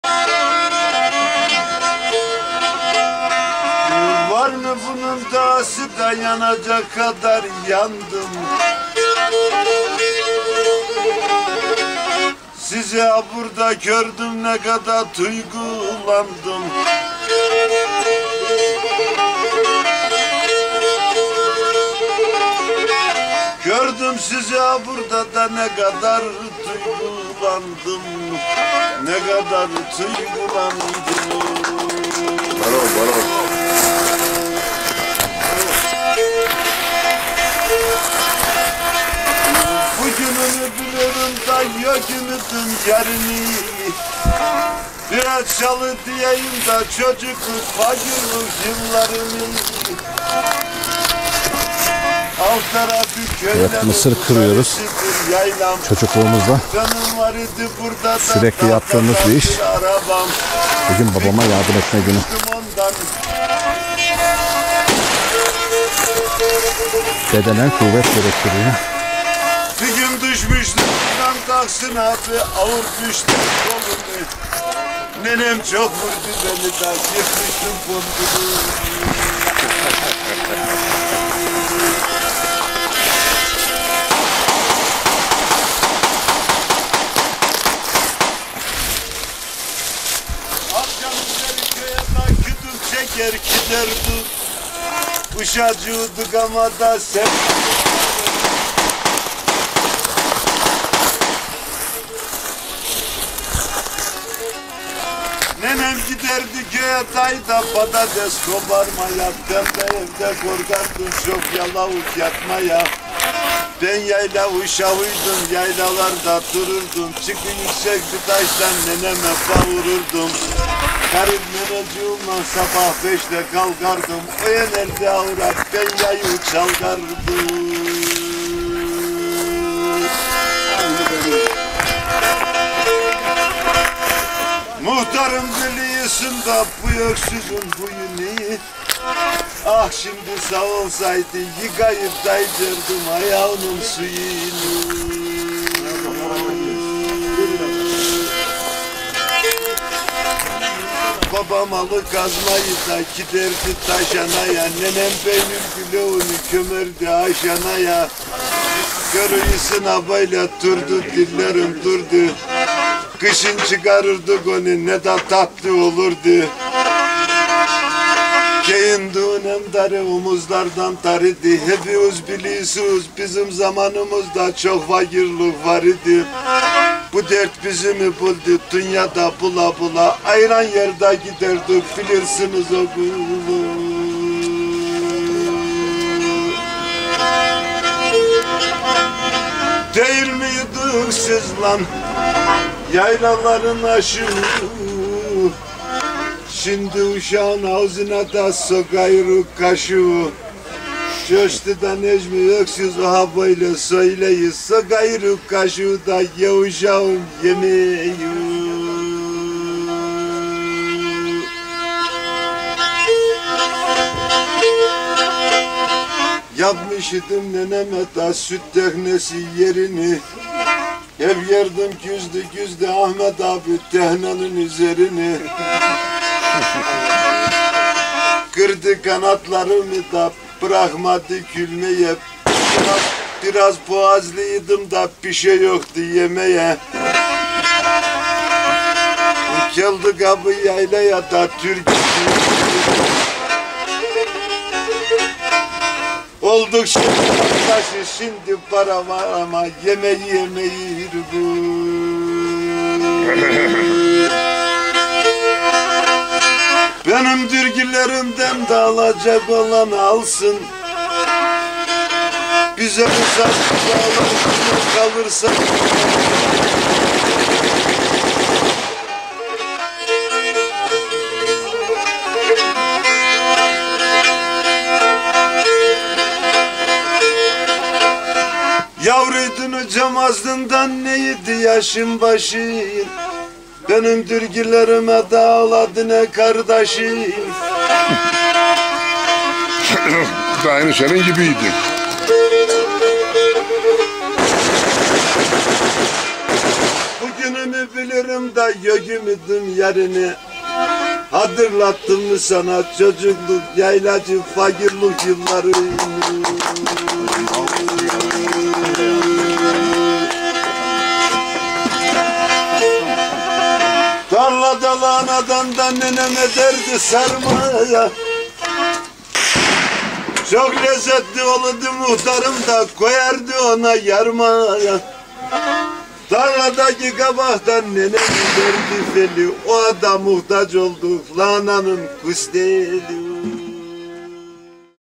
Mısır Nasıl Kırılır - Akçaabat Acısu Mah.
Trabzon Akçaabat Acısu Mahallesinden selamlar. Akçaabat Acısu (Koryana) Mahallesinde mısır kırma... İyi seyirler.